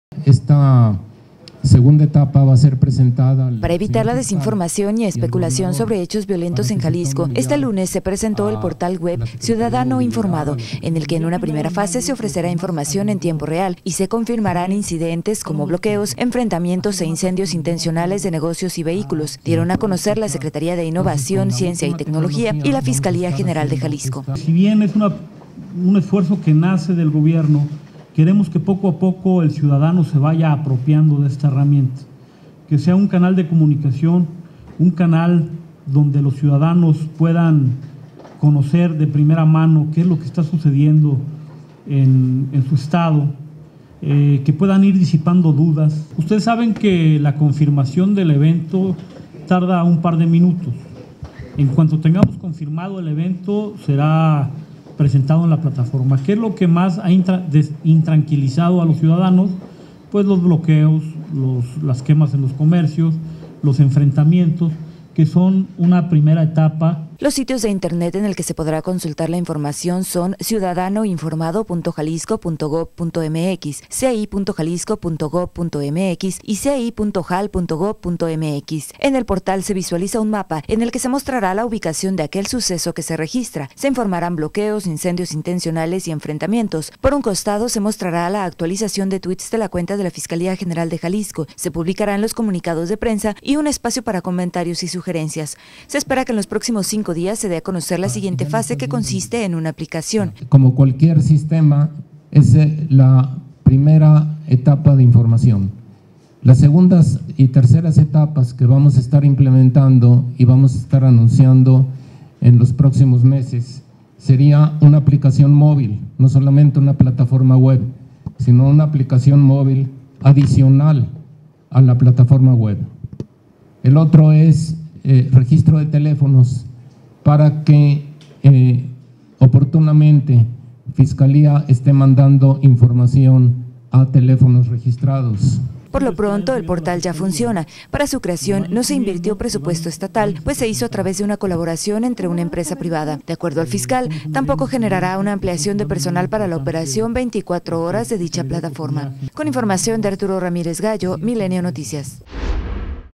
Lanzan plataforma para difundir información durante hechos violentos
Los sitios de internet en el que se podrá consultar la información son: ciudadanoinformado.gob.mx  ci.jalisco.gob.mx  y  ci.jal.gob.mx.